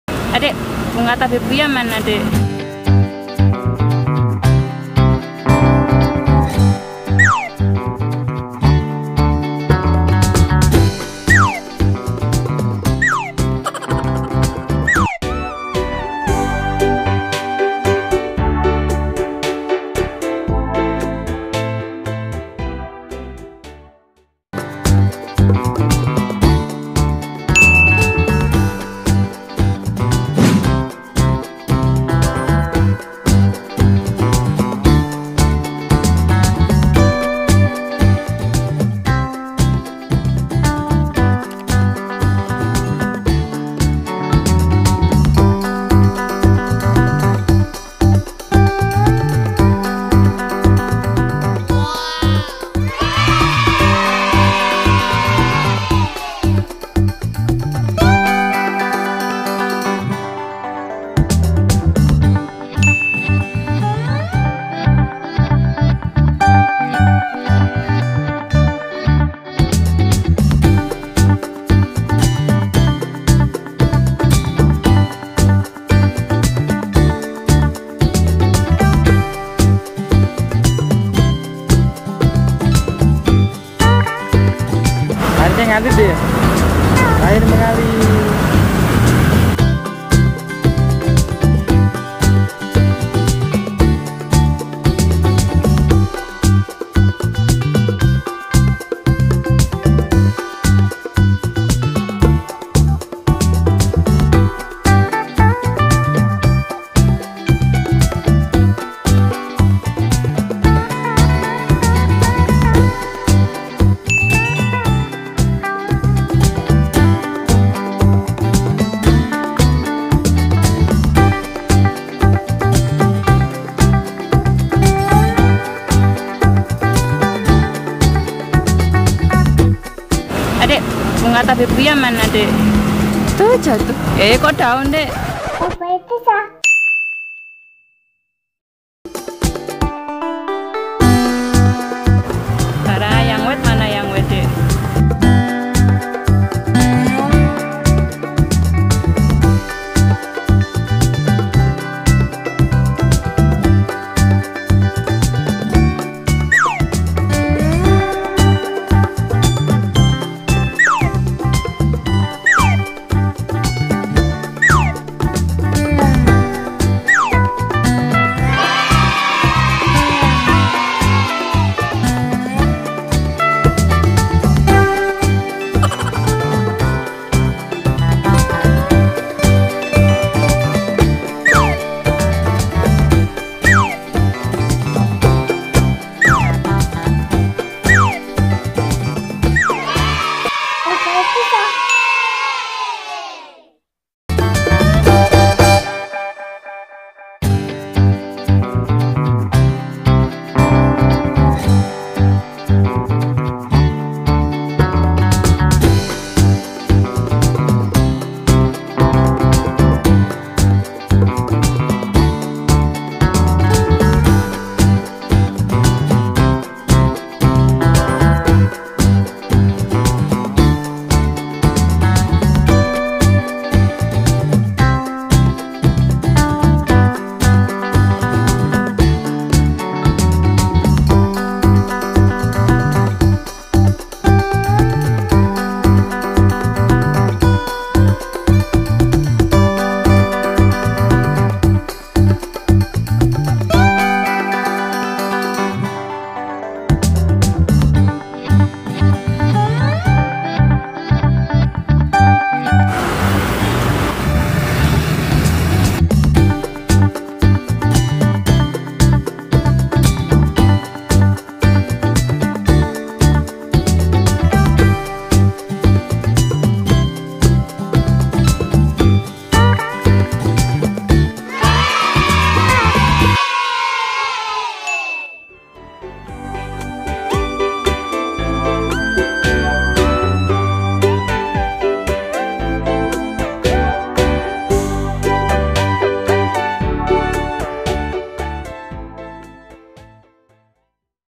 TABEBUYA BERSEMI || BEAUTIFUL BLOOMING TABEBUIA
Depan Kantor DPRD Kota Magelang

Masyaallah tabarakallah...

Assalamualaikum...
Halo teman-teman, Fara senang sekali diajak menikmati indahnya bunga Tabebuya yang sedang bermekaran. Tapi sayang sekali agak telat, jadi bunganya banyak yang udah rontok. Kalau di Korea ada cherry blossom, di Indonesia ada tabebuya blossom, yeayyyy. Semoga teman-teman semua terhibur...

Terimakasih...